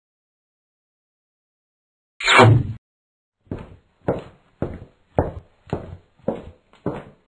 تجربة تحريك ببرنامج أنيميت Frame by frame Animation using Animate
تجربة مبدئية لعمل مشهد كارتون متحرك بسيط باستخدام برنامج أدوبي أنيميت